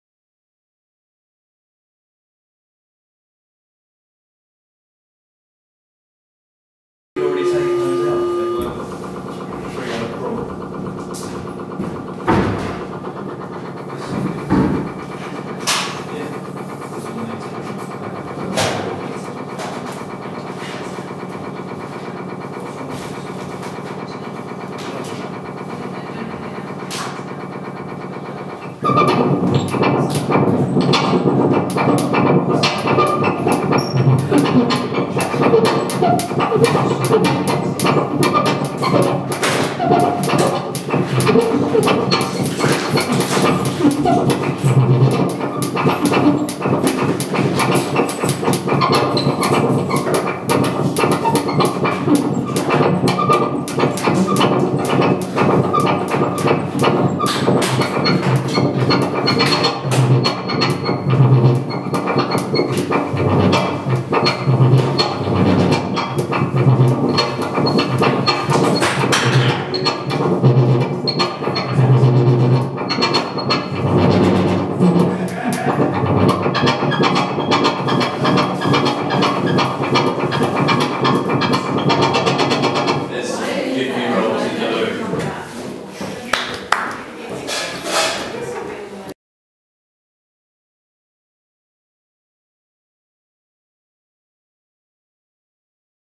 Martin Hackett
Martin Hackett testing the DRON at Hundred Years gallery, London E2 8JD
Saturday 23rd February, 2013